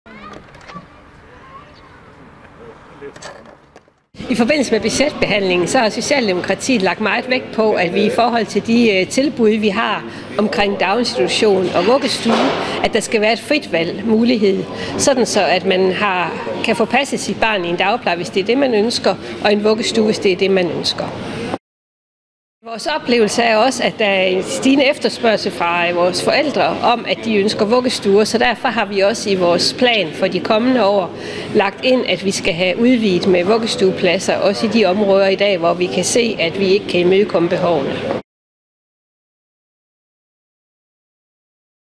Forældres frie valg
Udvalgsformand Lone Ørsted om forældres frie valg i Horsens: Om børnene skal i dagpleje eller vuggestue